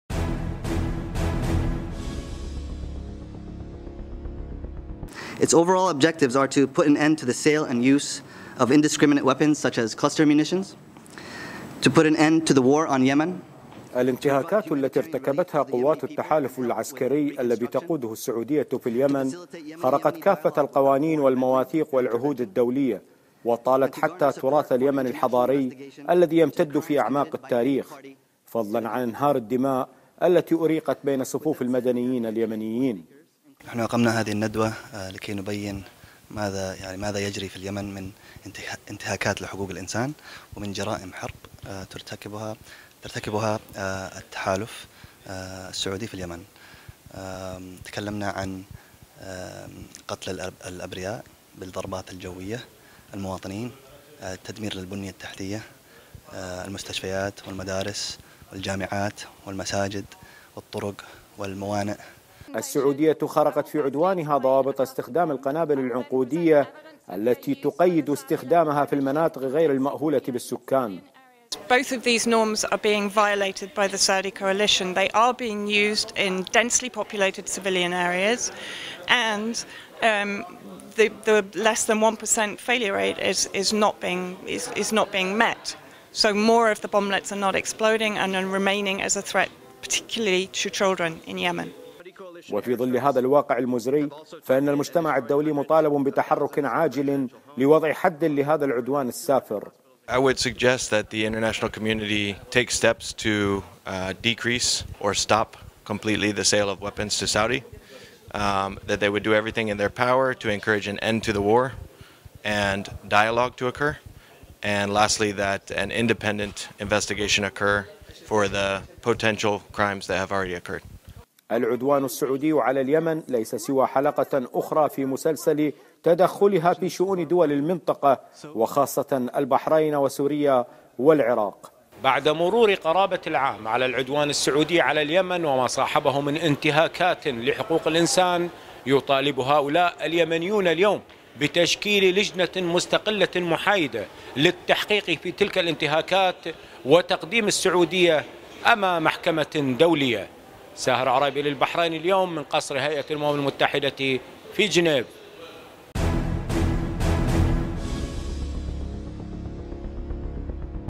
تقرير متلفز : السعودية تدمر اليمن وسط صمت دولي قاتل
البحرين اليوم ـ جنيف..

الإنتهاكات التي ارتكبتها قوات التحالف العسكري الذي تقوده السعودية في اليمن, خرقت كافة القوانين والمواثيق والعهود الدولية وطالت حتى تراث اليمن الحضاري الذي يمتد في اعماق التاريخ, فضلا عن انهار الدماء التي أريقت بين صفوف المدنيين اليمنيين.

السعودية خرقت  في عدوانها ضوابط إستخدام القنابل العنقودية  التي تقيّد إستخدامها في المناطق غير المأهولة بالسكان.

وفي ظل هذا الواقع المزري فإن المجتمع الدولي مطالب بتحرك عاجل لوضع حد لهذا العدوان السافر.

العدوان السعودي على اليمن ليس سوى حلقة أخرى في مسلسل تدخلها وعدوانها في شؤون دول المنطقة كالبحرين وسوريا والعراق وسوريا.